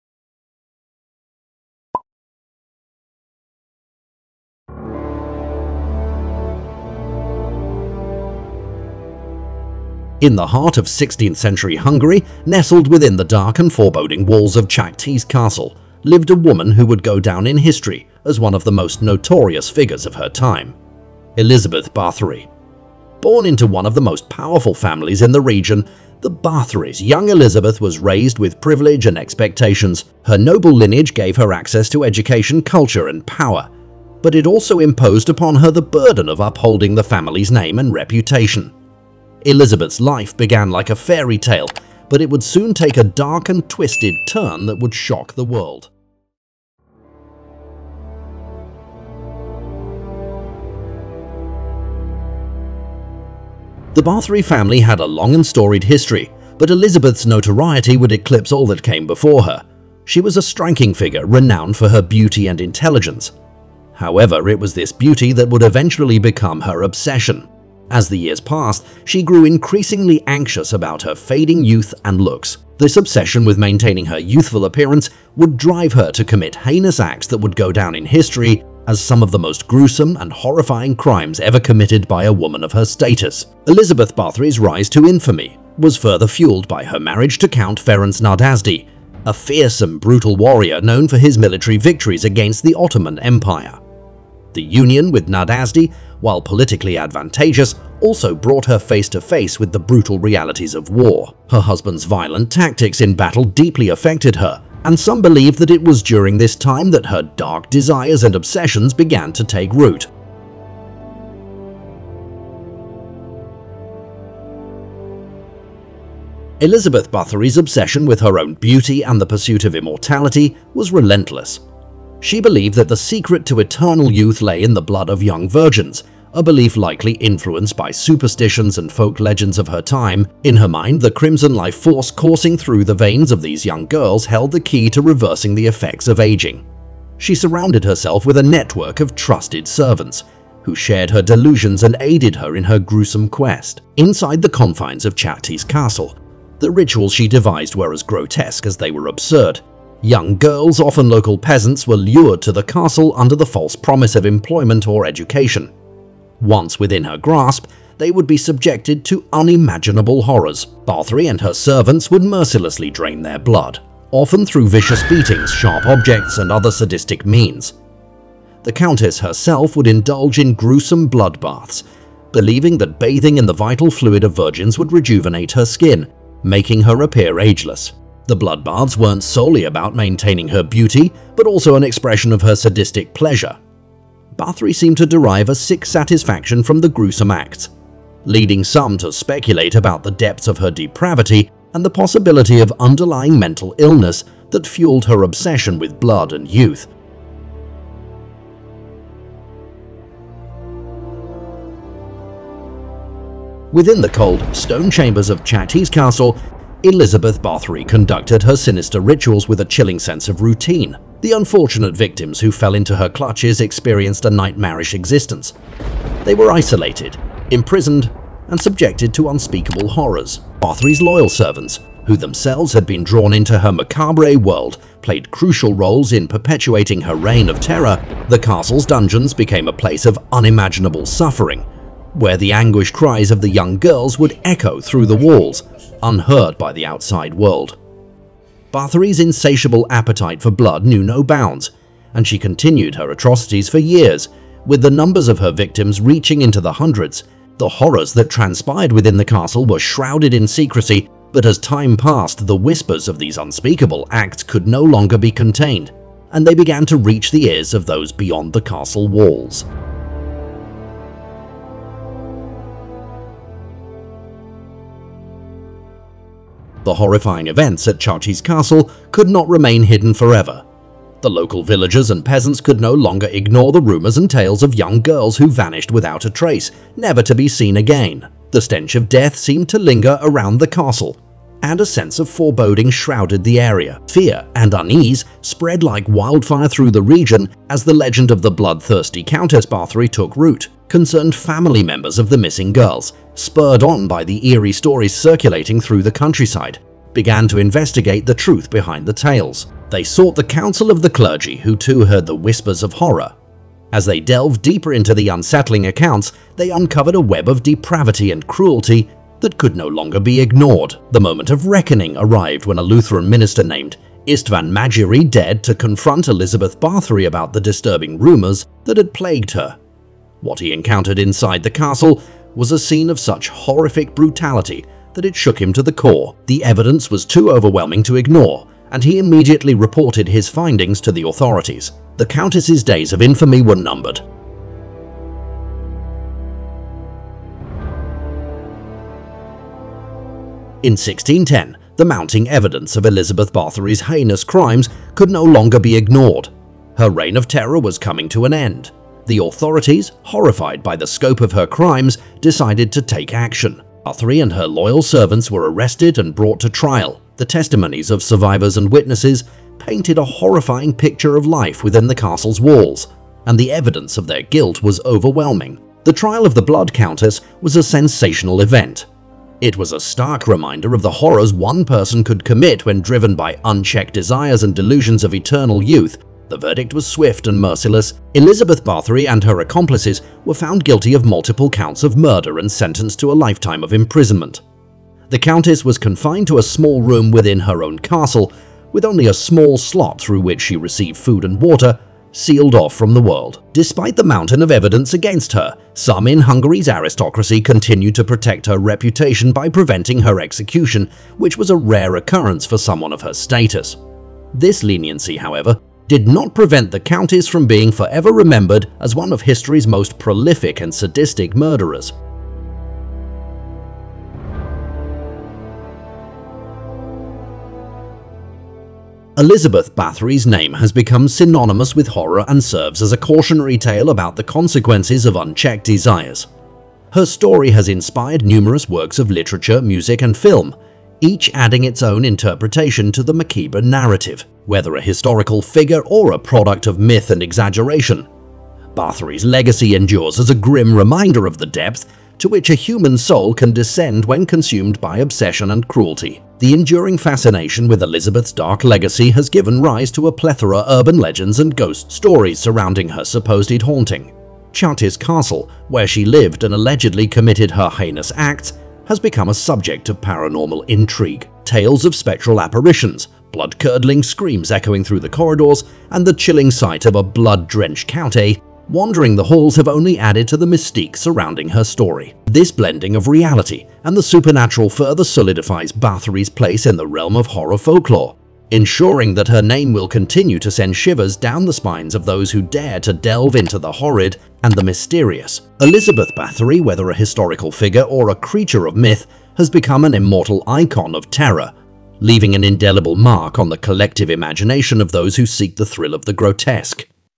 COUNTESS ELIZABETH BATHED IN THE BLOOD OF YOUNG VIRGINS
Uncover the chilling tale of Elizabeth Bathory in this video as we delve into the shadowy recesses of history to explore the life of a woman whose name is synonymous with terror. From the opulent halls of Čachtice Castle to the gruesome rituals that earned her the infamous title of 'Blood Countess,' we unravel the enigma surrounding her insatiable thirst for immortality.

CHAPTERS:
0:00 Intro
0:48 The Mysterious Countess
1:53 The Allure of Immortality
3:18 The Ghastly Ritual
4:02 Whispers of Horror
5:37 The Fall of the Countess
6:53 The Dark Legacy